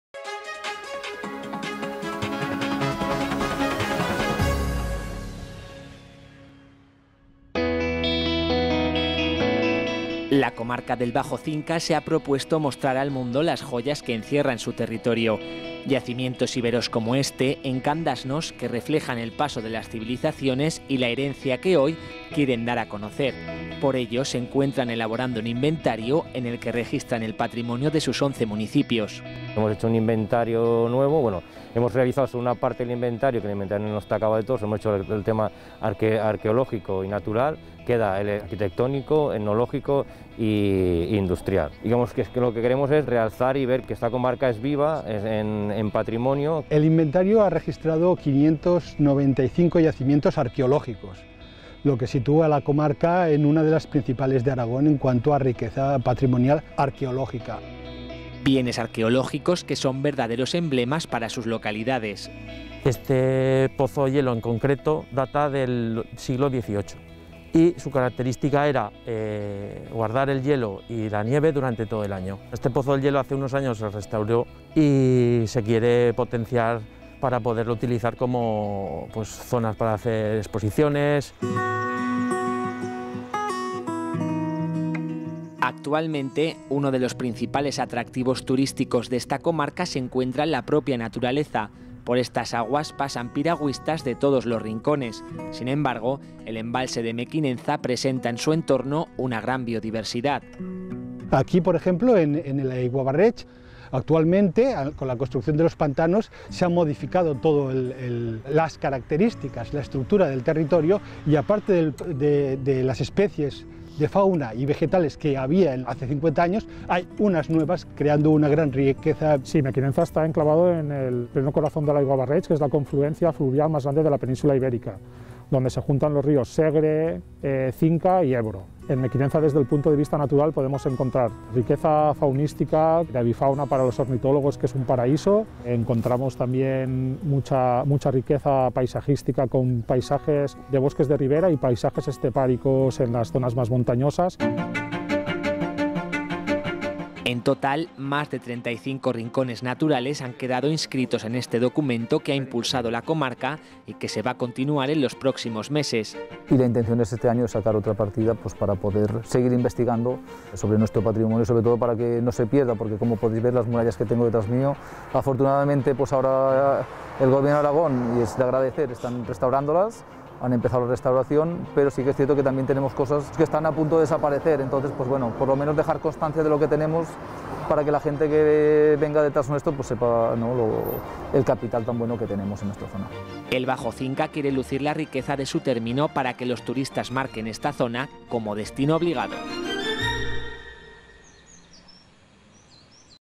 El patrimonio de la Comarca del Bajo Cinca
La Comarca del Bajo Cinca ha realizado un inventario del patrimonio arqueológico y del natural de todo el territorio comarcal, con la finalidad de dar a conocer estos bienes a la población para su valoración y conservación, y para lograr que los habitantes de la comarca sean actores principales en su protección.